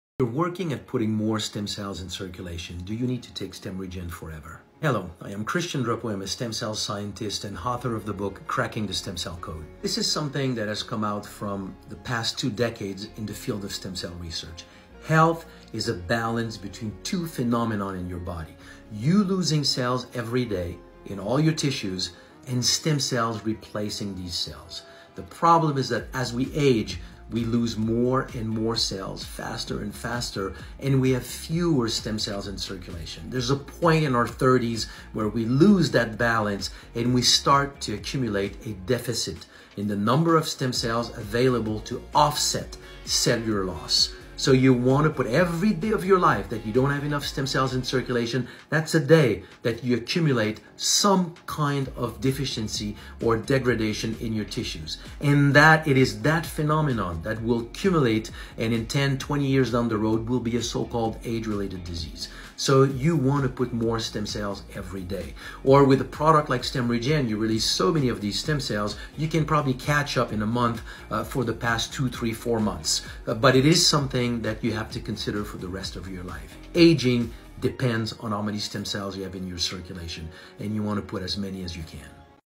Do You Need to Take STEMREGEN® Forever
Do you need to take Stemregen forever? Stem cell scientist Christian Drapeau, author of Cracking the Stem Cell Code, explains why maintaining stem cell circulation is essential for long-term health and aging. As we age, our body loses cells faster than it replaces them, leading to potential health decline. Stemregen helps boost circulating stem cells, supporting cell regeneration and overall well-being.

► Boost your body's natural repair system!  Try STEMREGEN® Release

Discover the science behind stem cell therapy, why cellular loss accelerates after your 30s, and how increasing stem cell production daily could impact your longevity and healthspan. Should you take Stemregen for life? Watch now to find out!